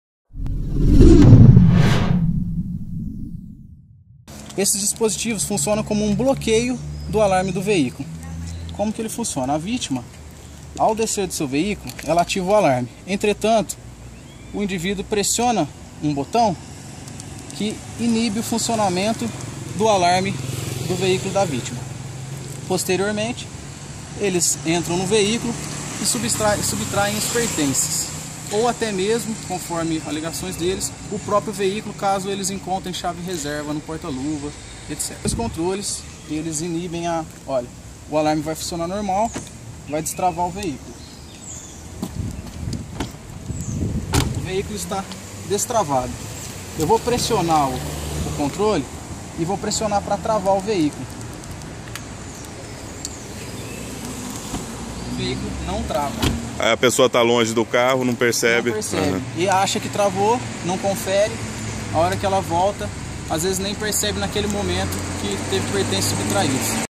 Dois são detidos por furto em Santa Bárbara
Dupla foi flagrada com dispositivo que desativa alarme de veículos; suspeitos estavam em posto na Rodovia dos Bandeirantes.